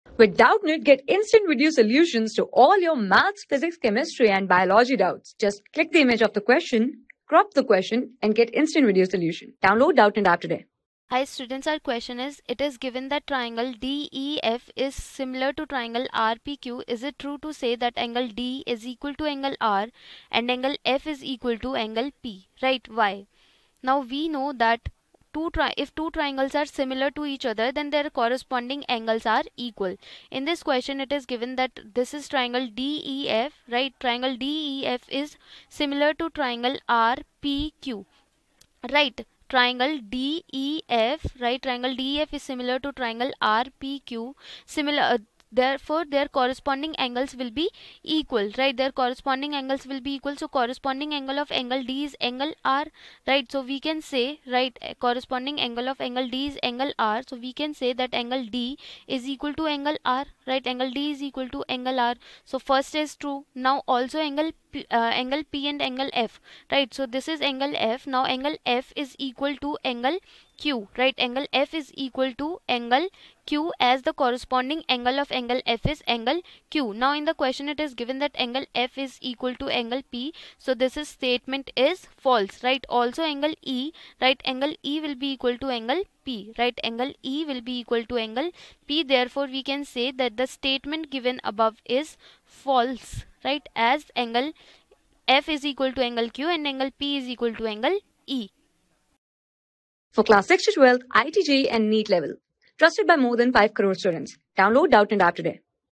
It is given that DeltaDEF~DeltaRPQ. Is it true to say that angleD = angleR and angleF= angleP? ...
It is given that DeltaDEF~DeltaRPQ. Is it true to say that angleD = angleR and angleF= angleP? Why?
Class: 10
Subject: MATHS
Chapter: TRIANGLES
Board:CBSE

👉 Have Any Query? Ask Us.